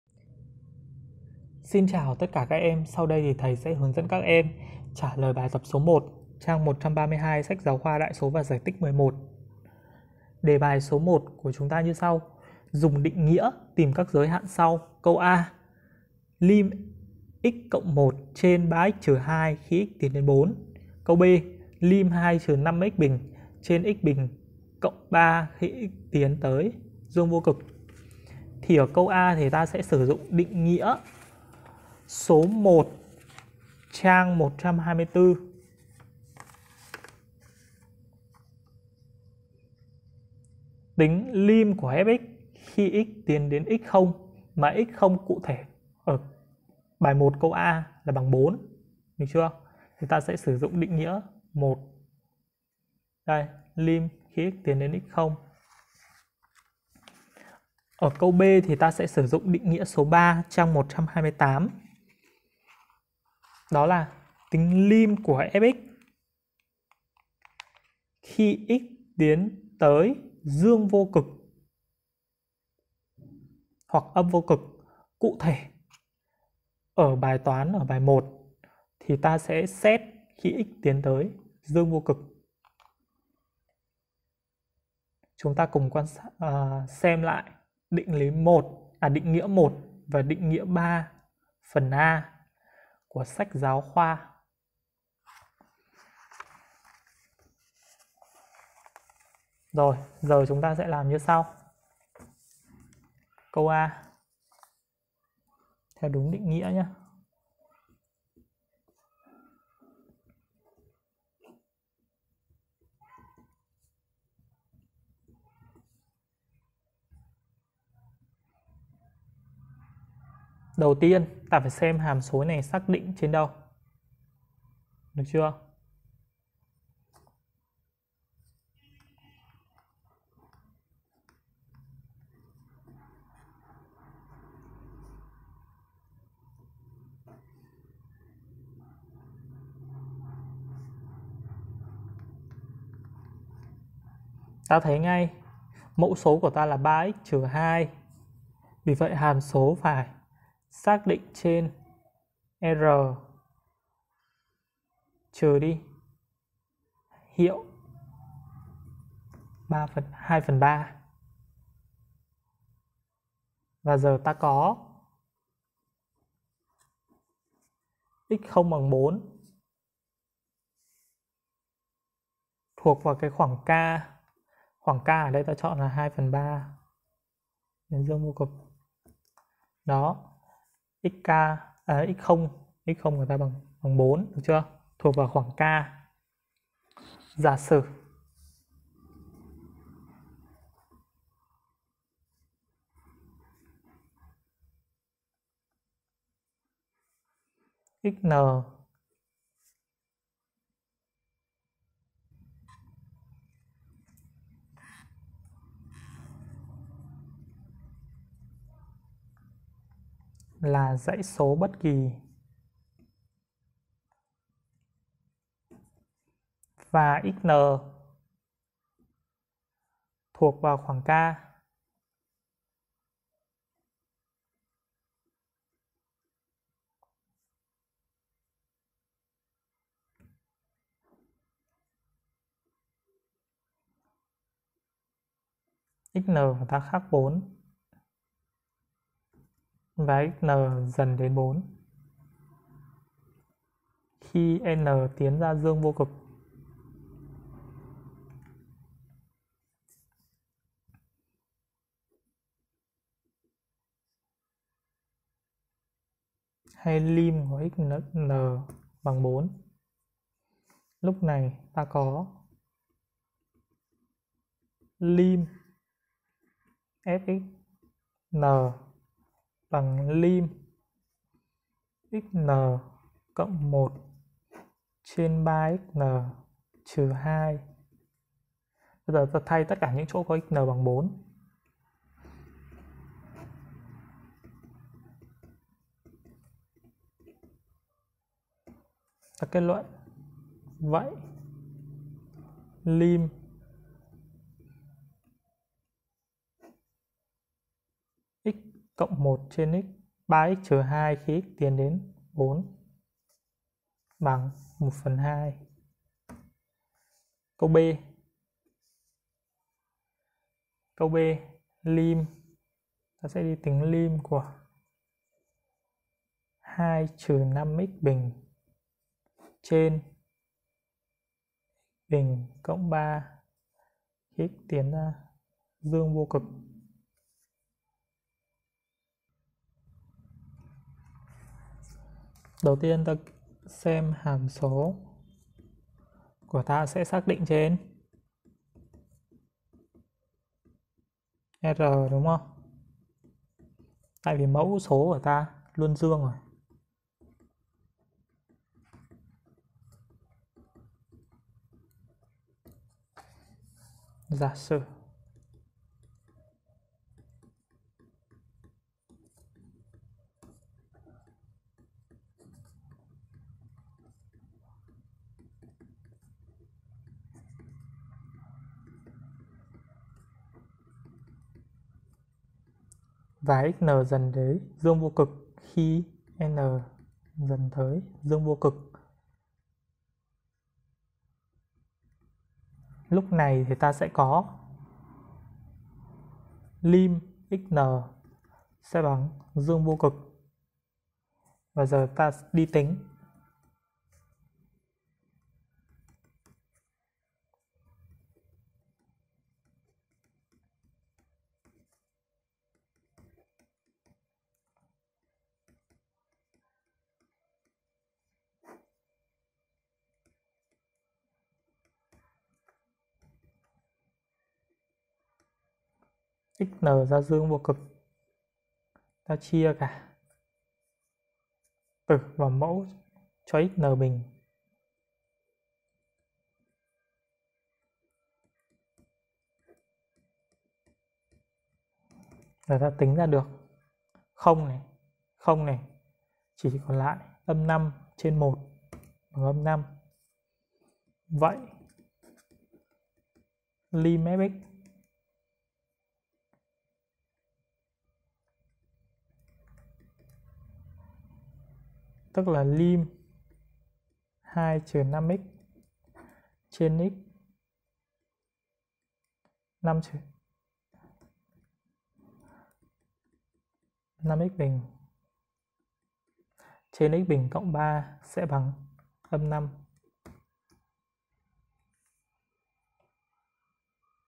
Giải bài 1 trang 132 SGK Đại số và Giải tích 11
Giải bài 1 trang 132 sách giáo khoa Đại số và Giải tích 11 với lời giải chi tiết, ngắn gọn nhất sẽ giúp các em nắm bắt các kiến thức cơ bản và nâng cao một cách nhanh nhất
Dùng định nghĩa tìm các giới hạn sau:
 a) \(\underset{x\rightarrow 4}{\lim}\dfrac{x+1}{3x - 2}\);
 b) \(\underset{x \rightarrow +\infty }{\lim}\dfrac{2-5x^{2}}{x^{2}+3}\). .